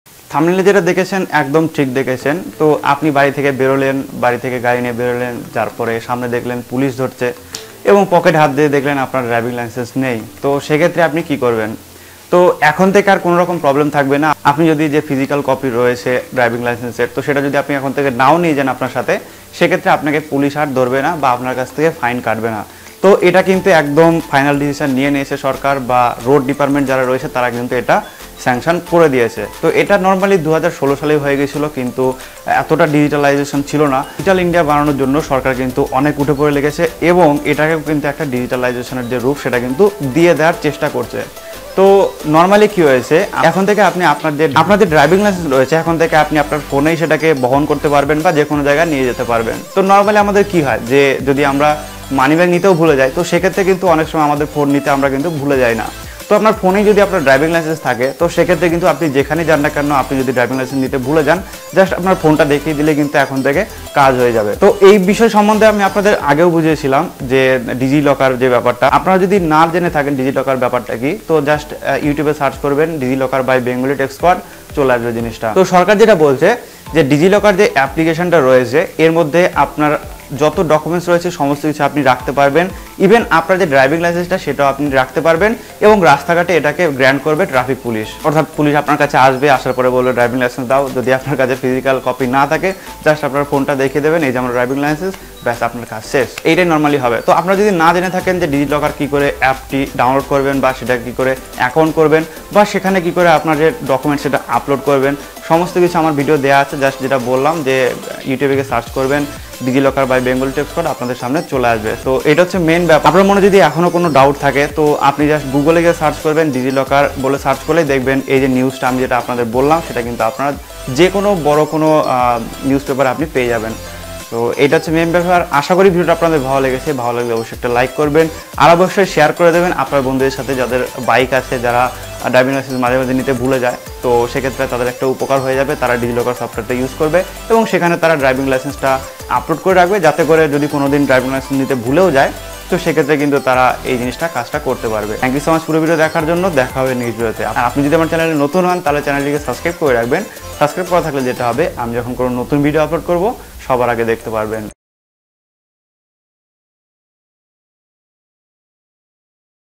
আর নিতে হবে না Driving License।কেন নিতে হবে না? সম্পূর্ণ তথ্য জেনে নিন।।
Bengali Techsquad